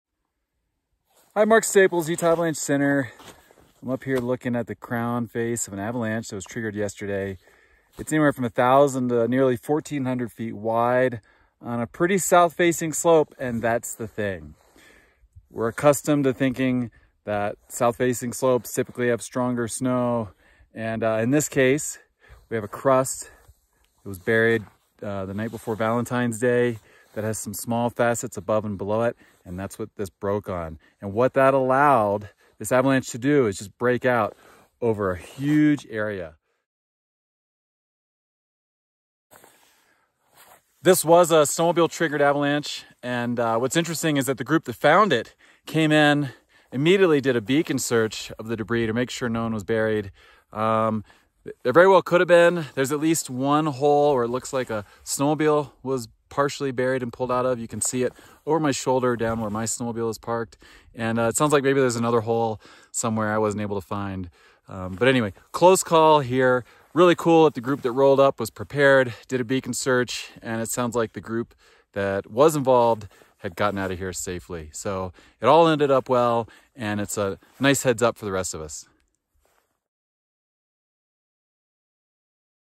1400 foot wide avalanche - 23 Feb 2024
Mark Staples investigates a snowmobile triggered avalanche in American Fork Canyon that happened the day before. It was 1400 feet wide and about 2 feet deep failing on a thin layer facets above an ice crust on a south east facing slope.